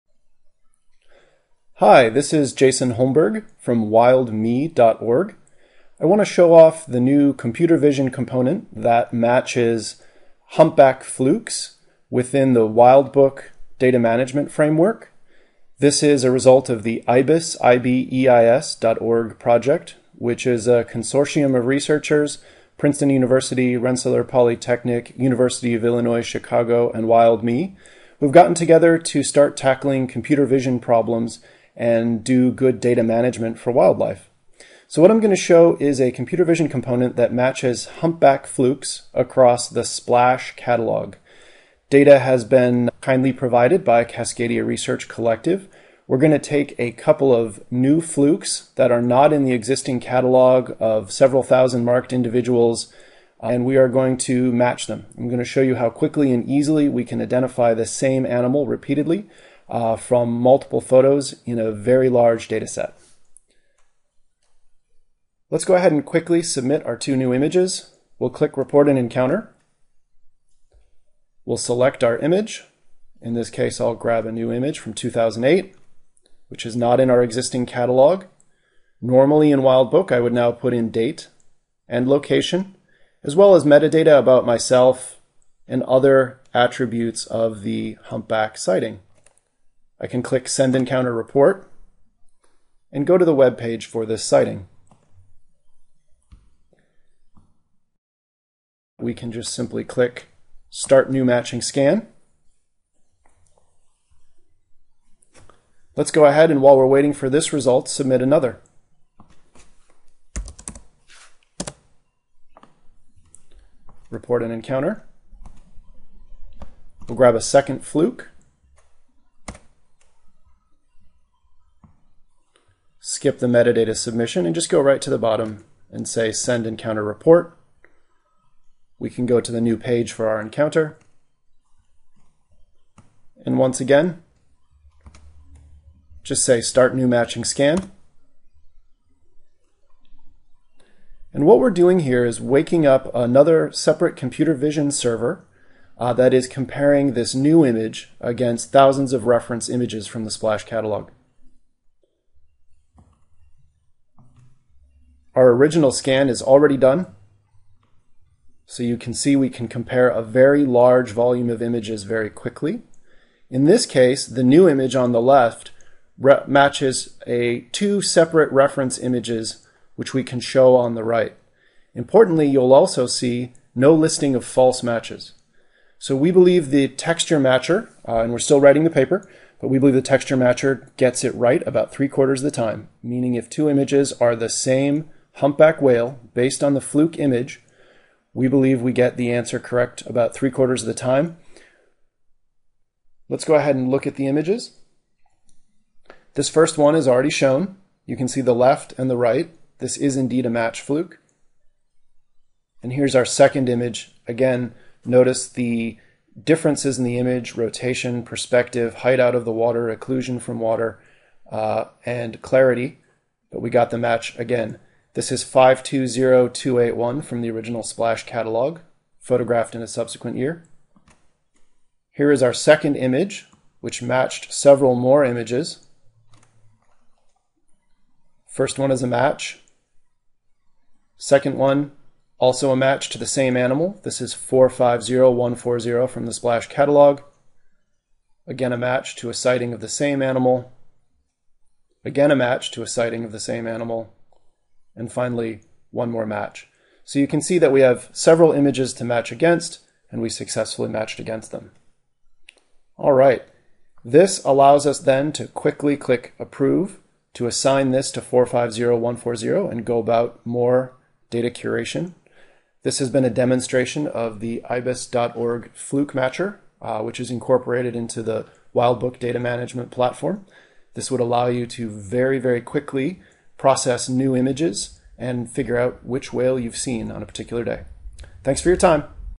Humpback Fluke Matching with Wildbook
This is a demonstration of applying Wildbook Image Analysis (computer vision developed under the IBEIS.org project) to matching humpback flukes in mark-recapture. Reference images are matched against the SPLASH catalog. Data used with permission of Cascadia Research Collective.